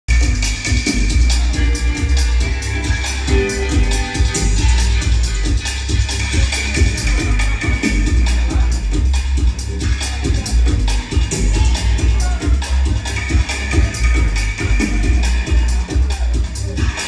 Freedom Masses sound system plays a tuff dubplate 4 _ 05/2016
DUB SCHOOL Sat 7th may 2016
FREEDOM MASSES meet MA-KAYA
Manchester & Birmingham meet in Bedford
Sound system favourites Freedom meet longtime Midlands Kings Makaya @ Six's Night club